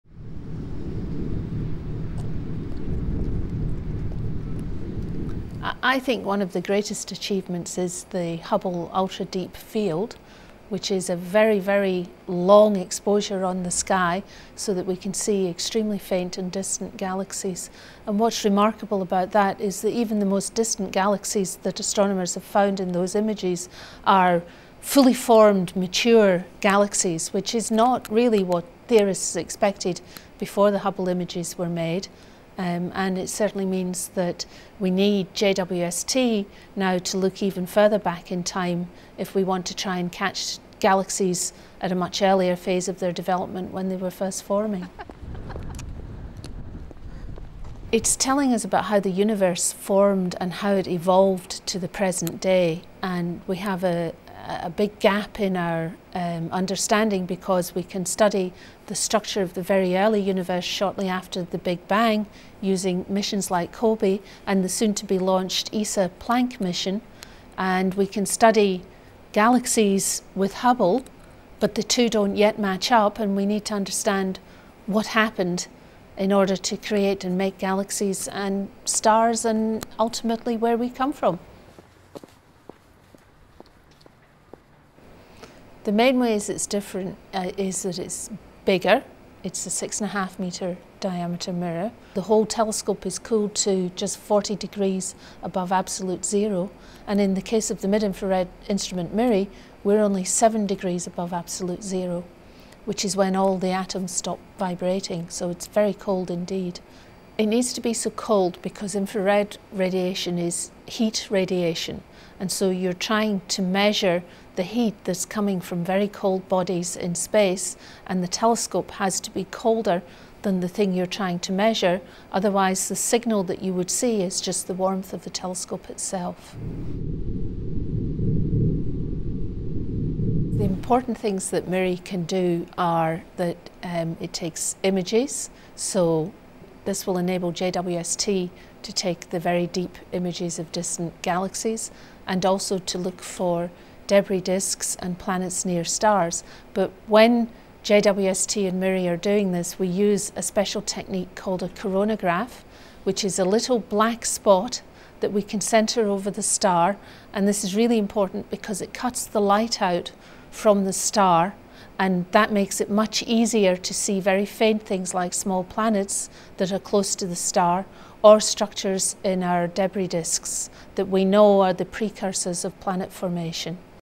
Why are Hubble's discoveries so important?
What has Hubble achieved and how is JWST different from Hubble? Gilian Wright, European Principal Investigator for the MIRI instrument on James Webb, explains.